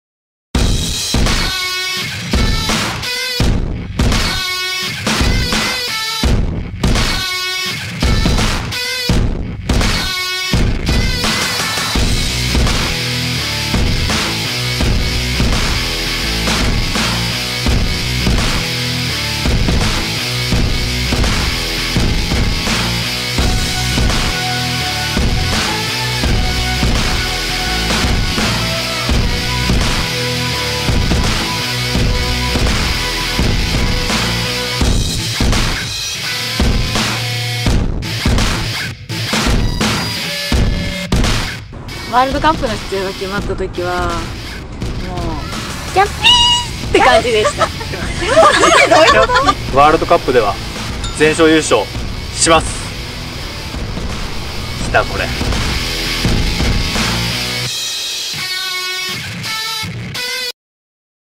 HADO WORLD CUP 2017 FINALISTS 7 -- わちゃわちゃ☆ピーポー
自らの手で技を放て！
AR（拡張現実）で実現するテクノスポーツ「HADO」

賞金総額300万円！「HADO」の世界一を決めるHADO WORLD CUP 2017が12月3日に開催決定！